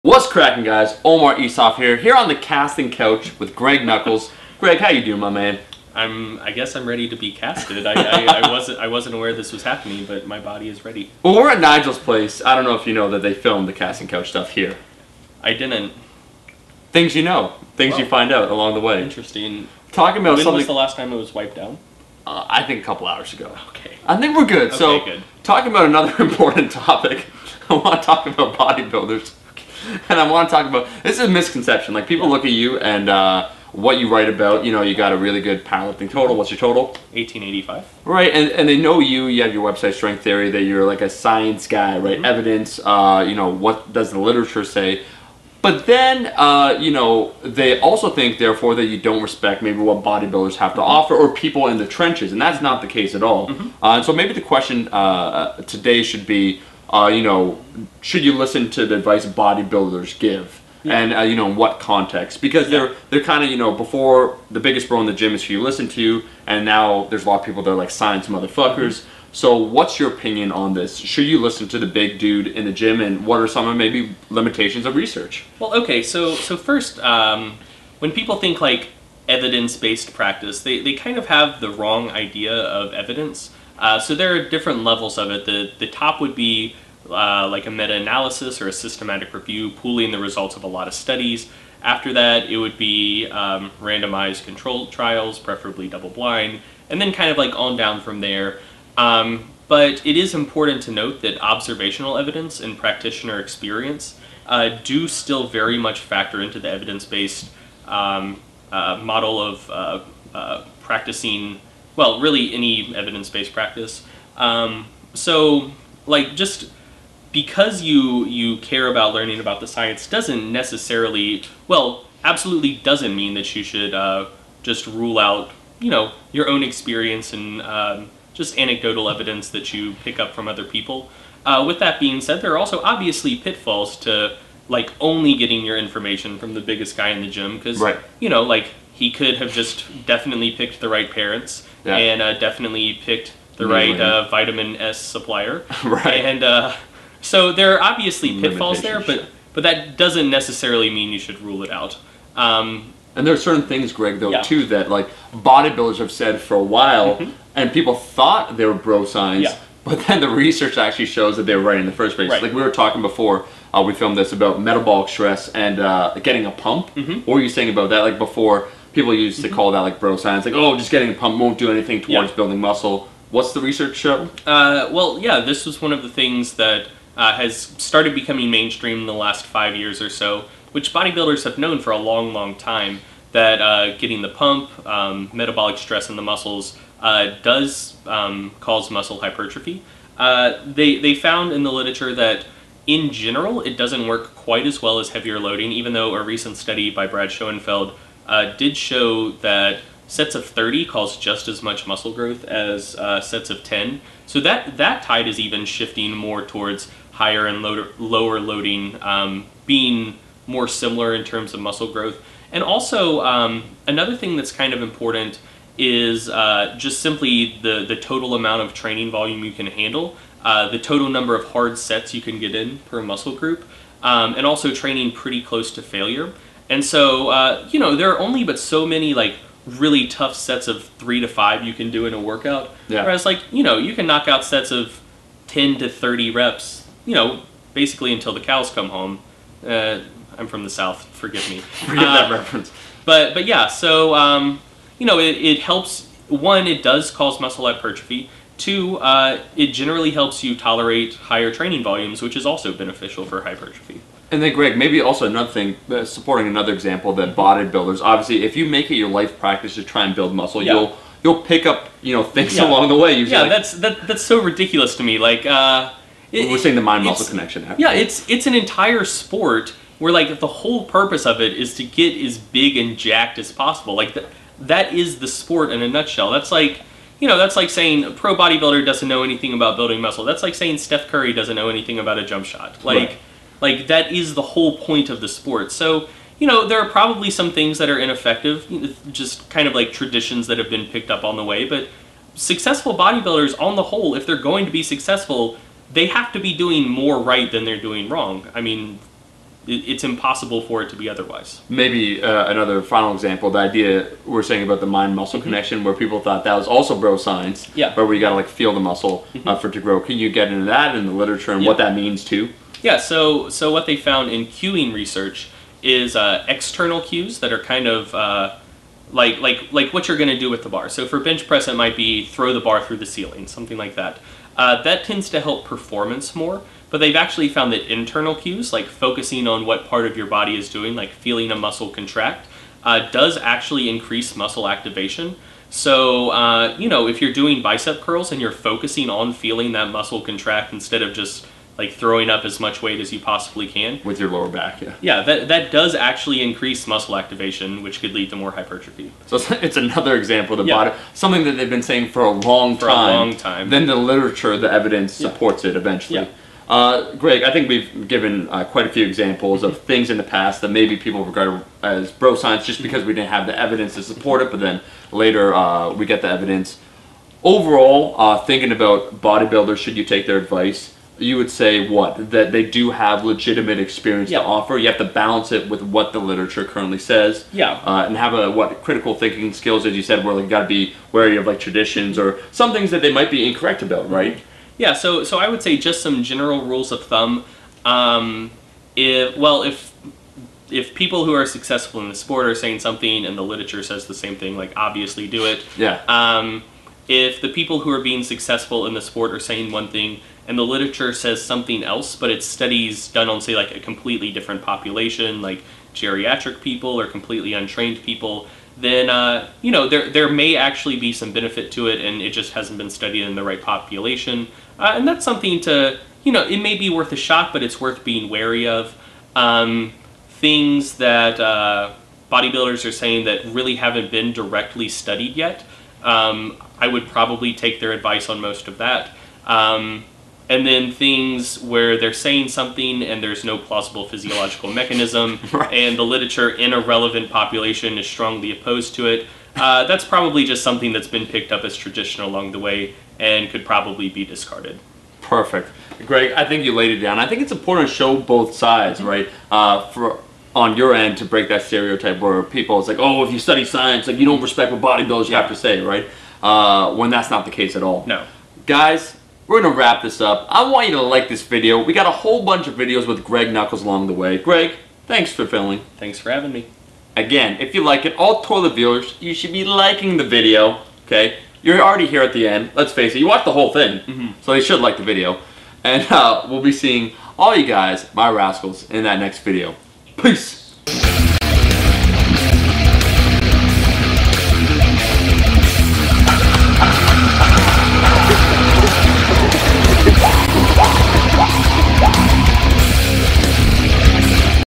Should You Take Lifting Advice From Bodybuilders?
Here's a writeup of brad's recent ~10 rep vs. ~30 rep study from the man himself (link to the actual study